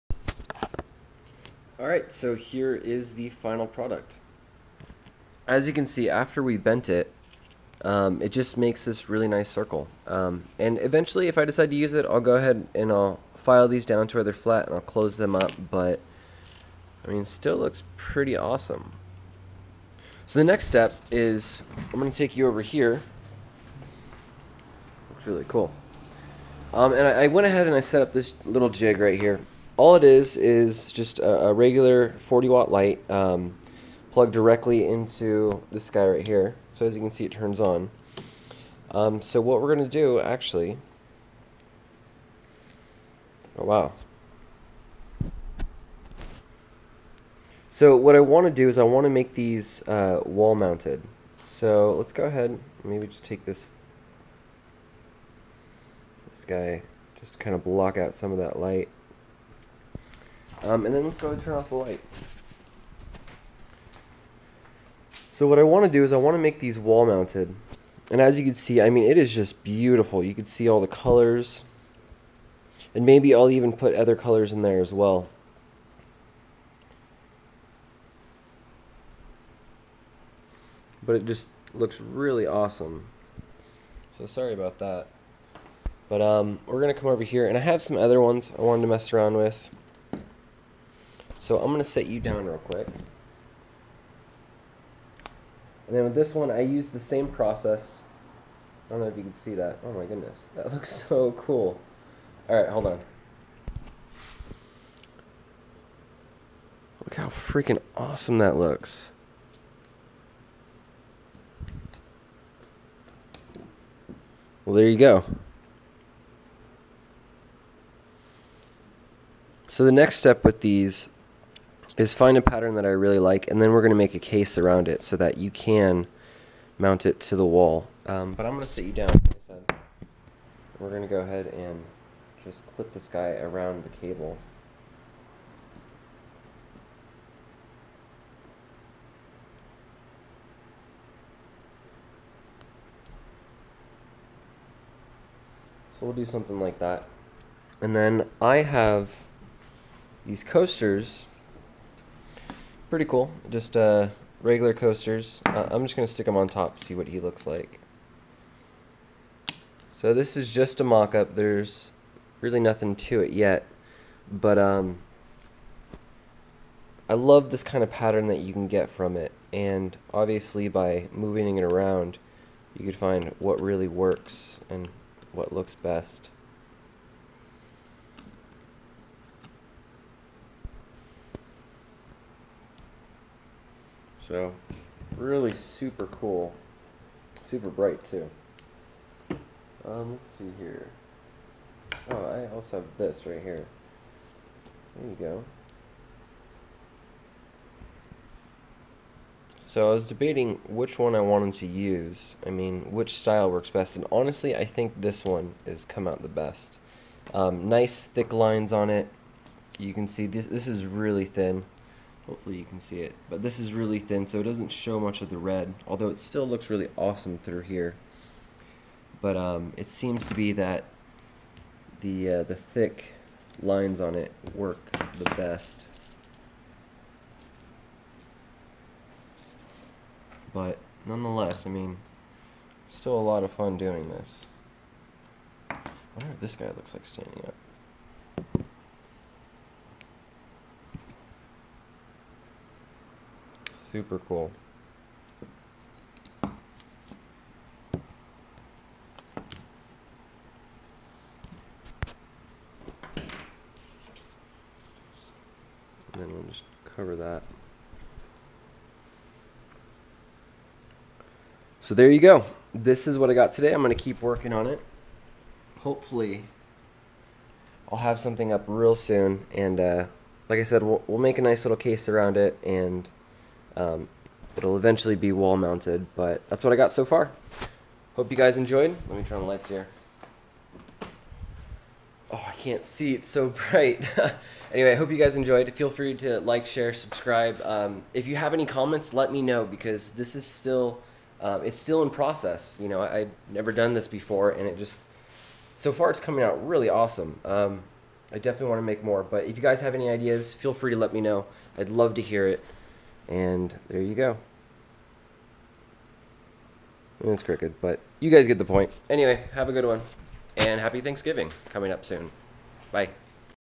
SHADOW ART
working on some ideas for a wall mounted light up design.